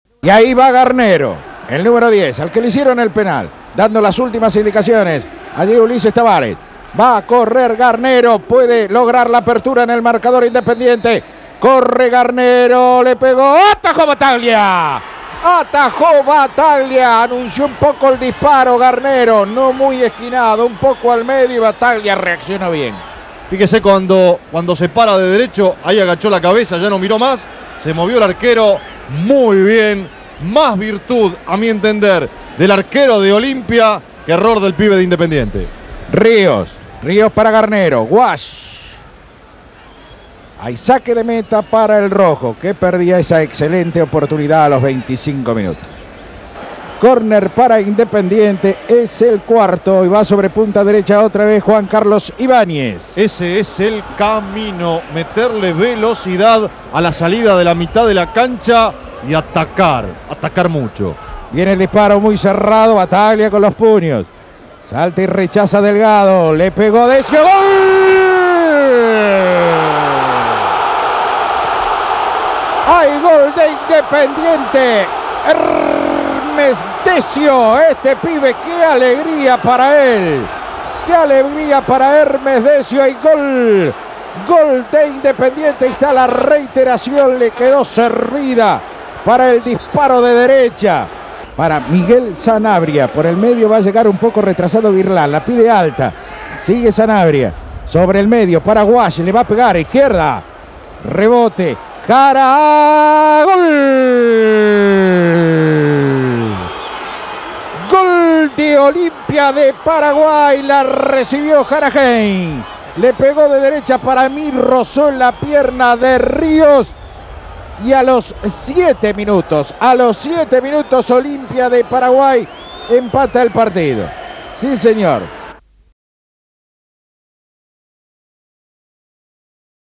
DiFilm - Independiente vs Olimpia de Paraguay 1 a 1 (1991)
Battaglia le ataja un tiro penal a Garnero. Arbitro Ulises Tavares.
Fecha: 17/10/1991
Duración: 2 minutos 5 segundos.
Código: UG-2868